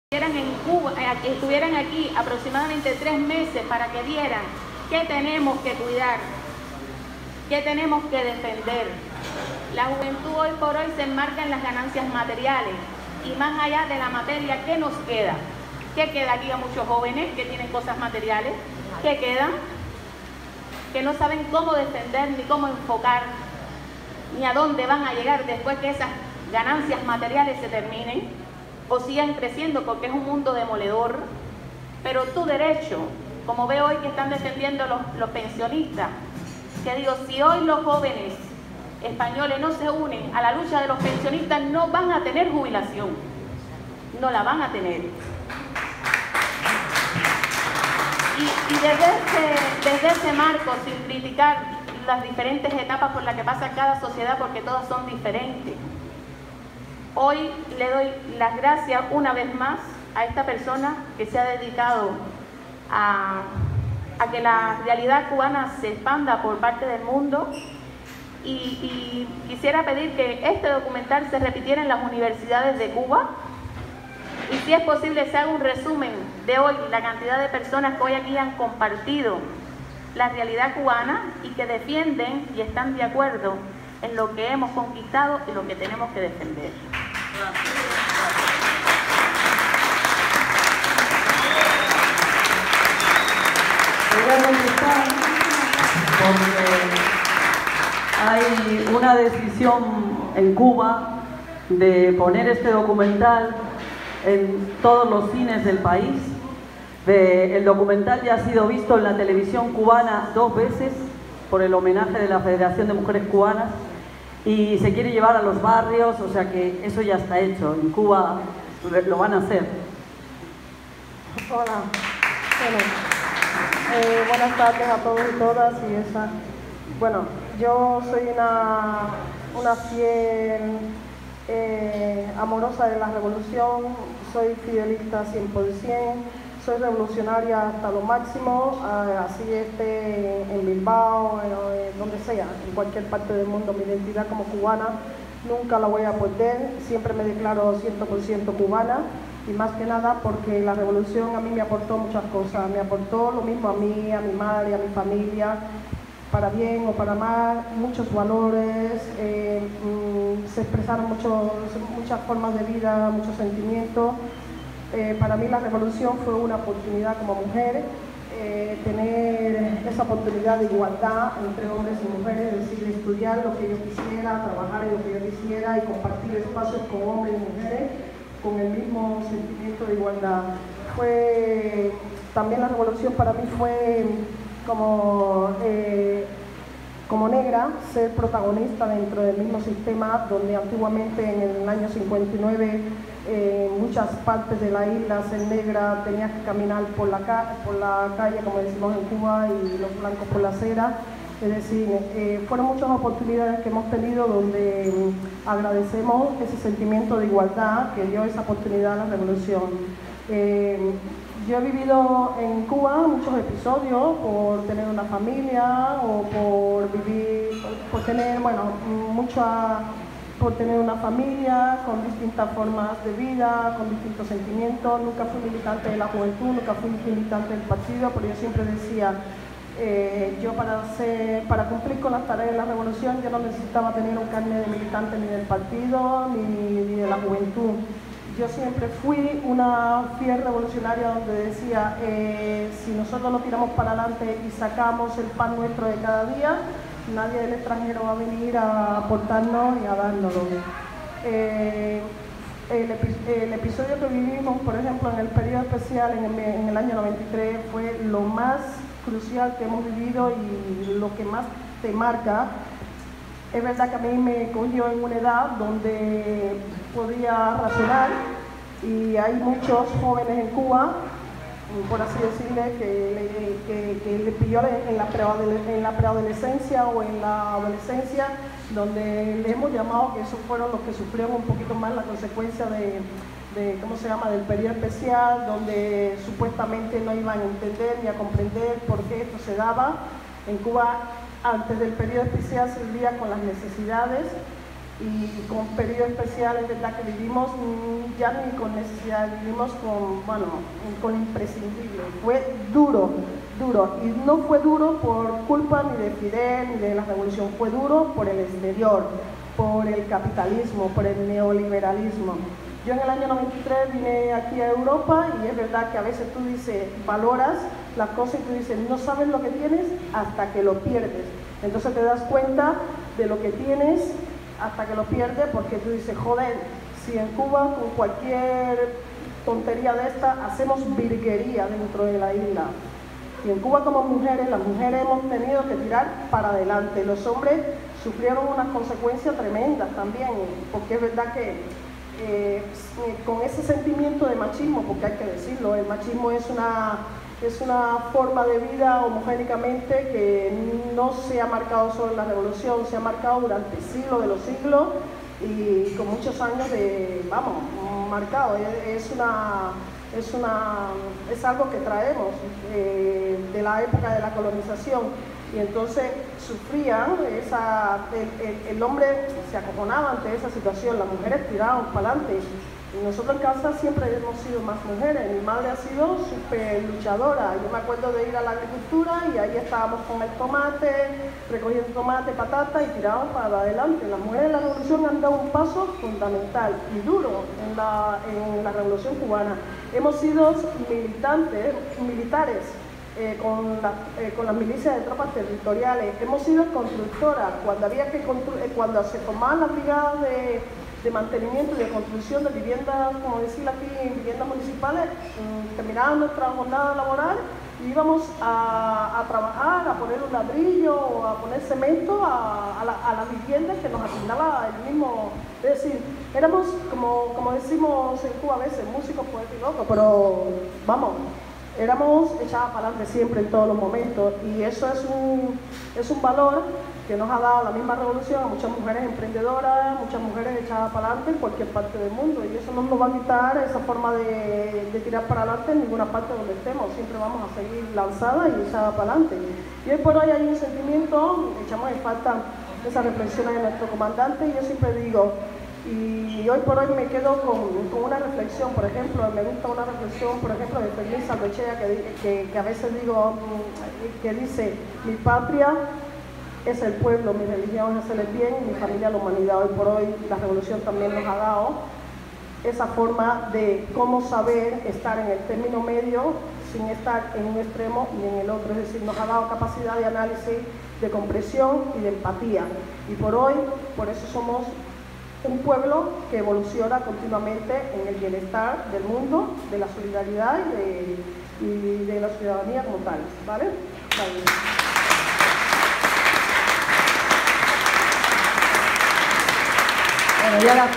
Cubanas, Mujeres en Revoluciòn en Bilbao, hablan las cubanas
Las participantes de la Agrupaciòn Sierra Maestra de Bilbao, cubanas residentes en Bilbao, hablan despues de ver la pelicula  Cubanas, Mujeres en Revoluciòn.